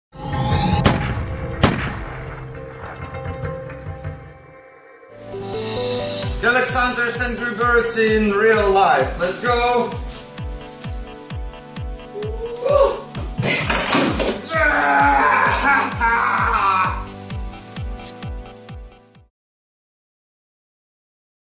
ScavHunt2016 Angry birds IRL is better than app from Galaxunters
scavhunt2016 we played angry birds in real life and it was awesomeee...

No actual brids were harmed)